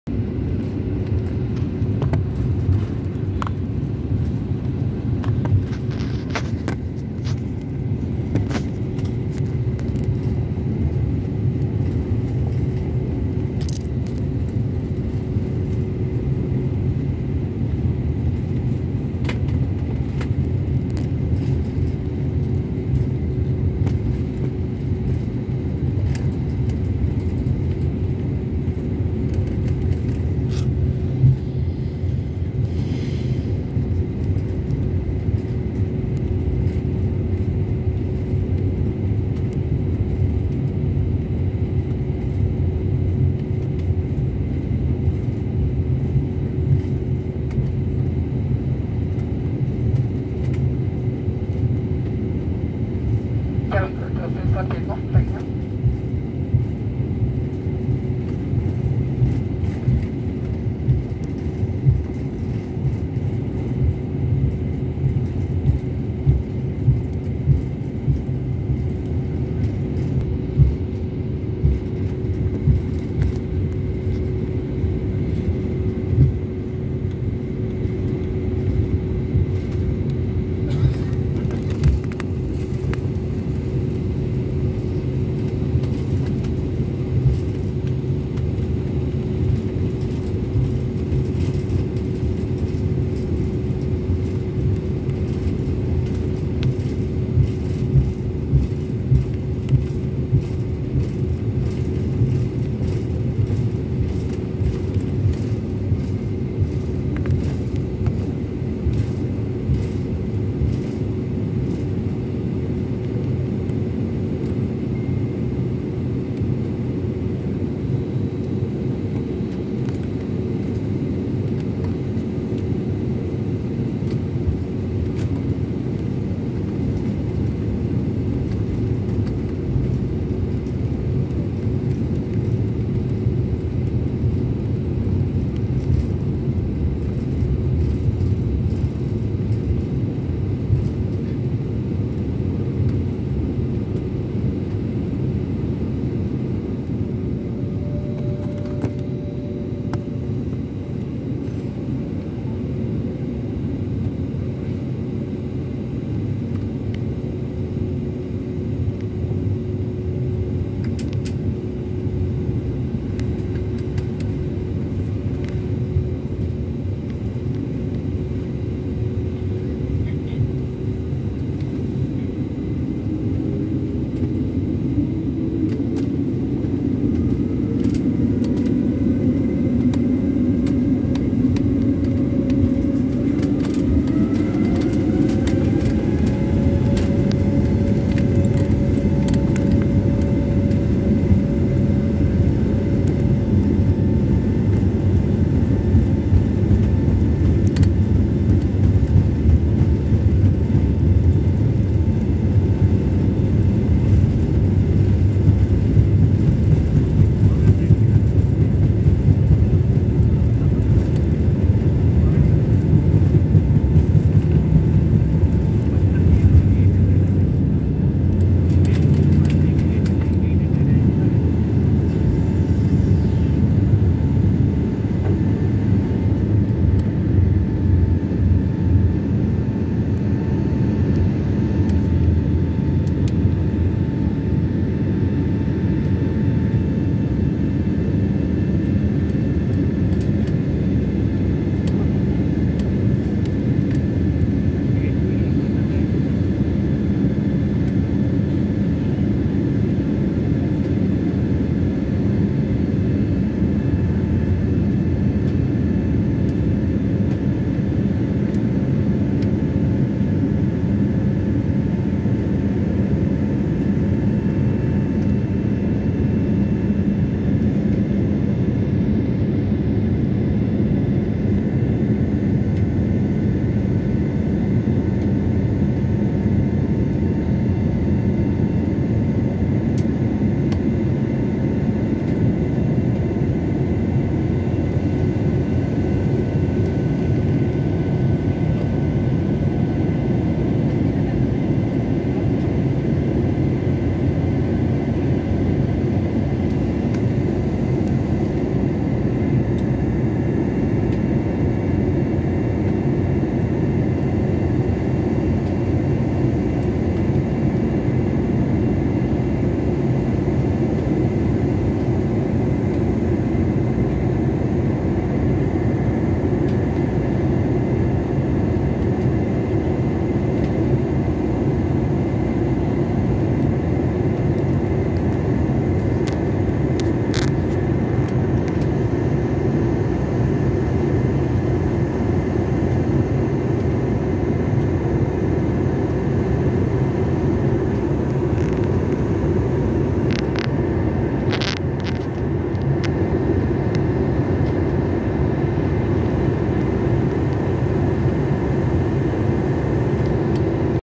Flight take off view at Bengaluru international Airport. Bangalore to Ahmedabad.
Beautiful aerial view of flight take off from Bangalore international Airport  and also early morning sunrise view...
Bangalore to Ahmedabad.